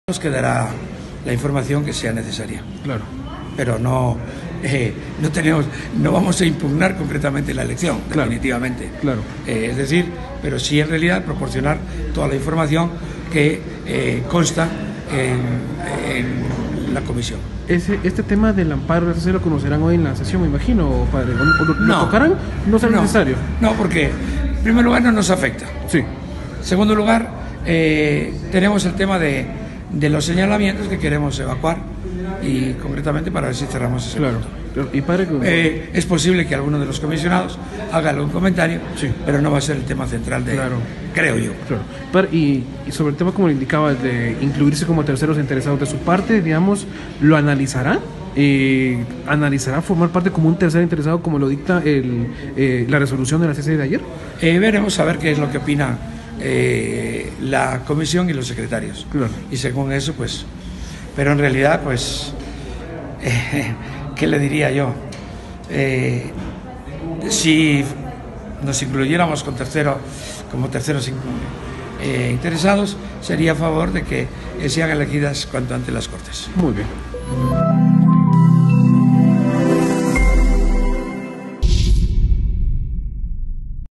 Serrano: de ser terceros interesados, abogaríamos porque se elijan magistrados
El presidente de la Comisión de Postulación para la elección de magistrados de Corte Suprema de Justicia (CSJ), Félix Serrano, dijo que de ser terceros interesados abogarán para que el Congreso elija a los nuevos magistrados.